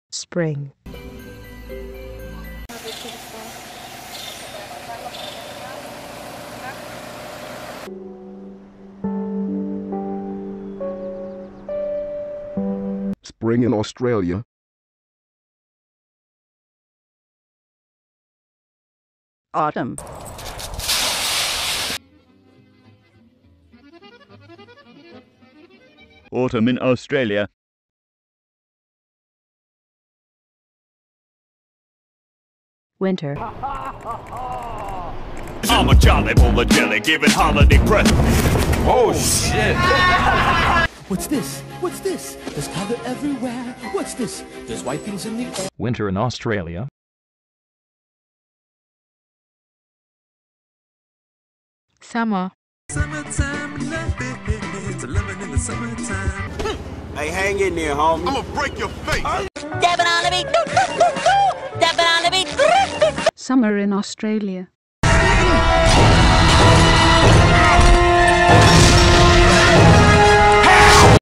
Australian Seasons vs (Mostly) Everyone Else's Seasons
It just came into my head one day to make this video. Now that I think about it, I'm probably gonna get a lot of copywright law handed to my ass.
SPRING AUTUMN WINTER SUMMER (Who cares if they're not in order)
As an Australian, I know that we don't experience different seasons, we just know it's a different season. The more you know :D